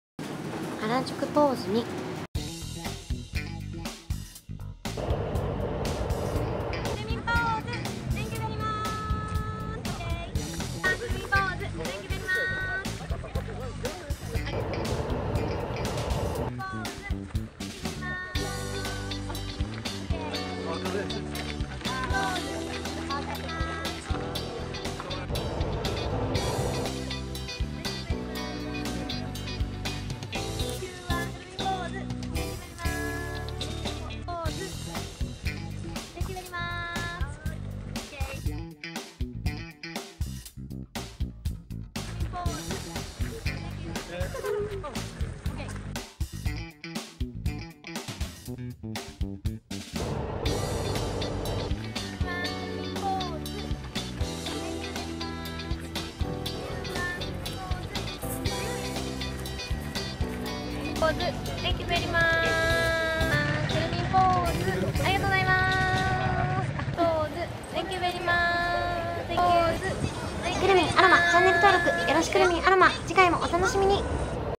ハロウィンメイクポーズ集2明治神宮のピエロ？あだ名は暴走自転車？パワースポット
皆さん楽しいポーズをありがとうございましたみん！
原宿おしゃれな皆さん☆☆本当に素晴らしい☆風邪予防にも効くるみんアロマポーズ☆